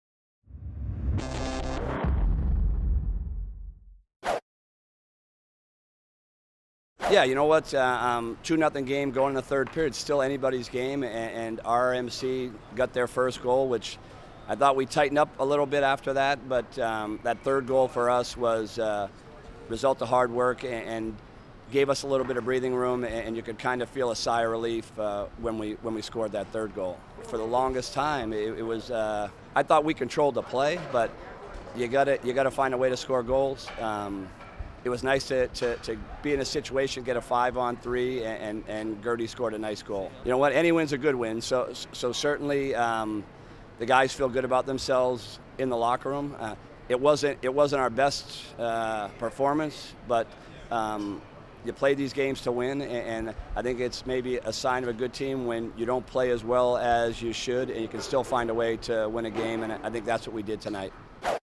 v RMC 1-26-13
Army scores a 4-1 win over Canadian foe RMC.